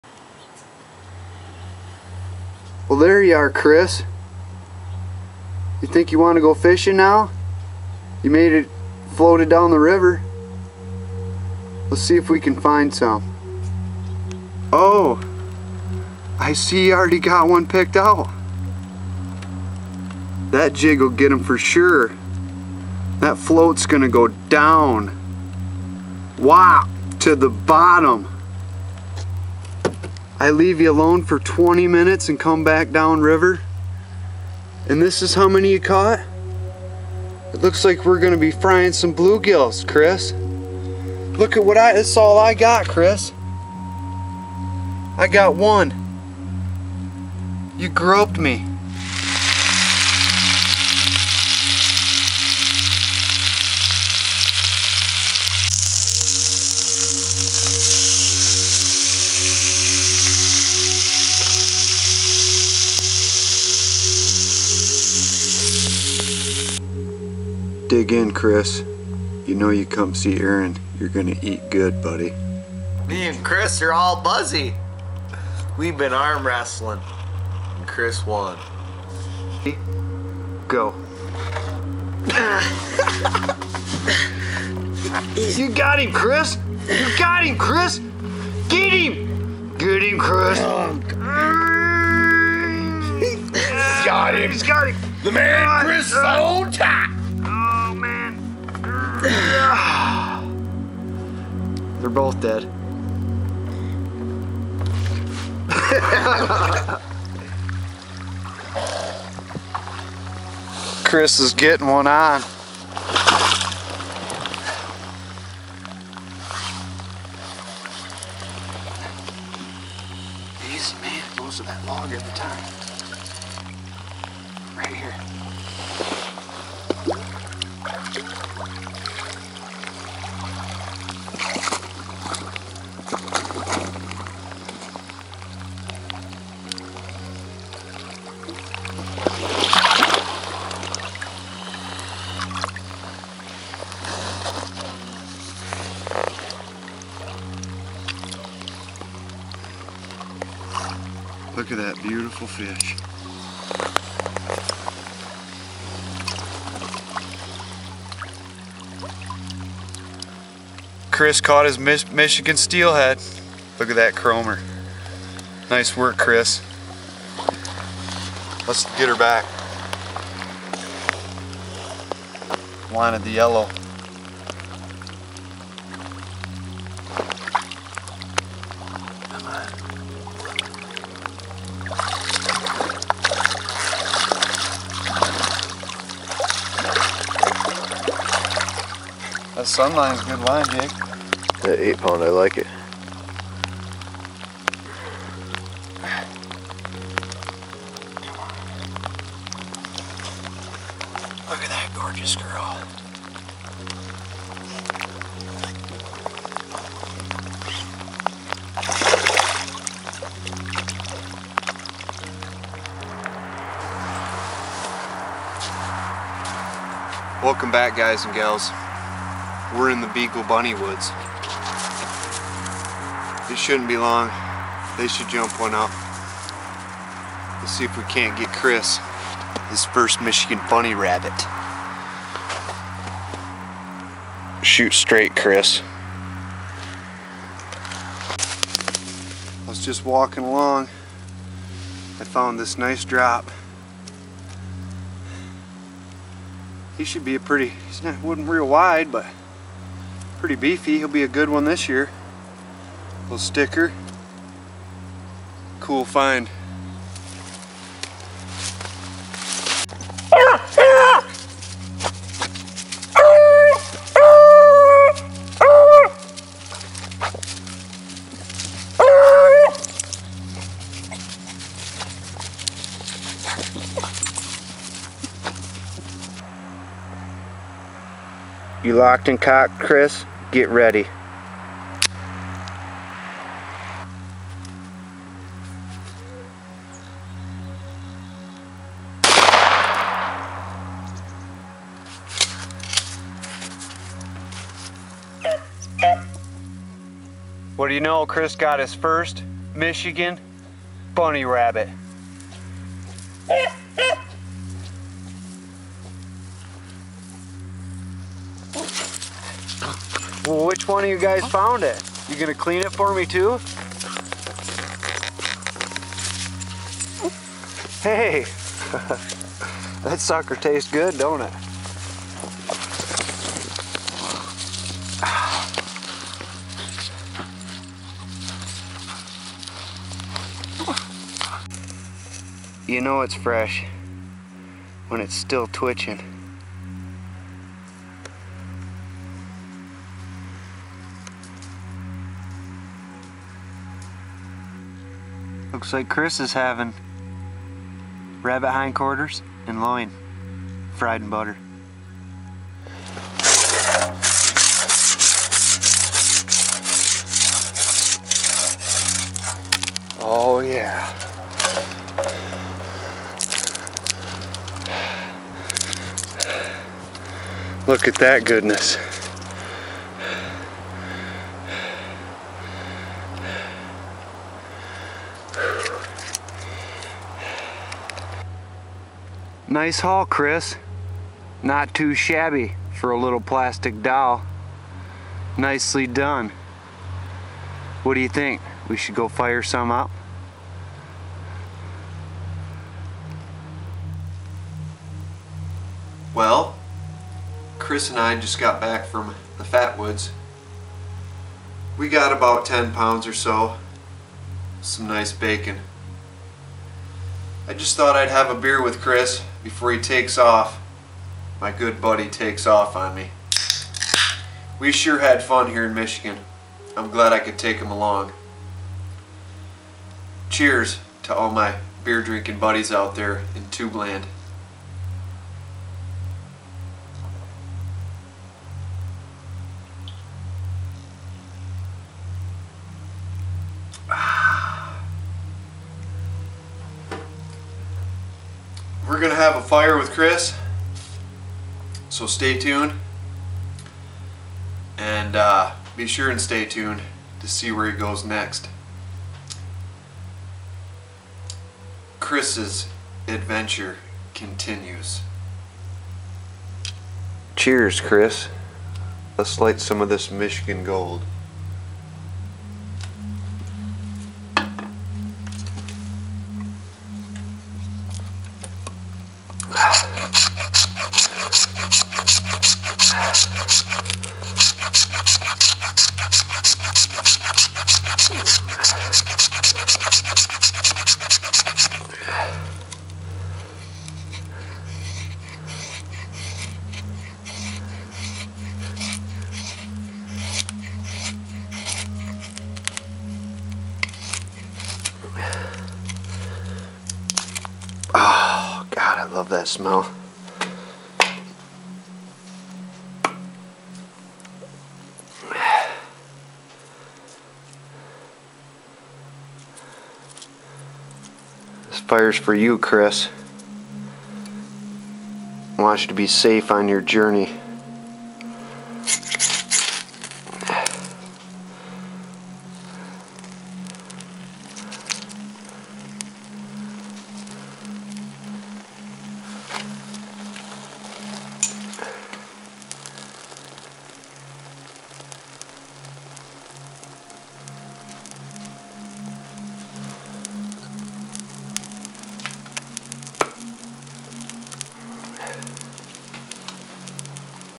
Mid Michigan Mission
Just doing what I do! Enjoying the Outdoors! Glad to take Kris along with! He has been around the world! We had fun on our Mid Michigan adventure! Huh wonder where he will go next for some fun? Thanks for coming along my friends!! Get out and have fun!!